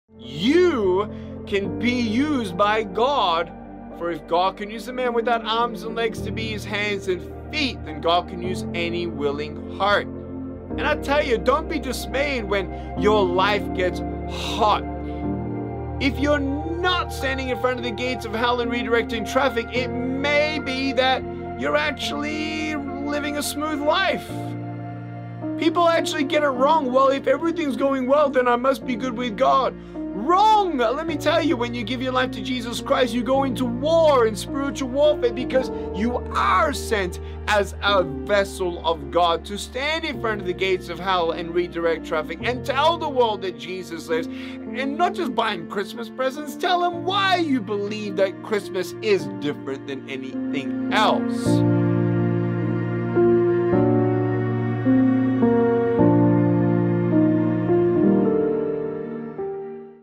The Cost of Being God’s Vessel with Nick Vujicic
"The Cost of Being God’s Vessel with Nick Vujicic" means that you stand in front of the gates of hell and redirect traffic. Being used by God as His vessel of salvation to mankind will not be easy but definitely all worth it!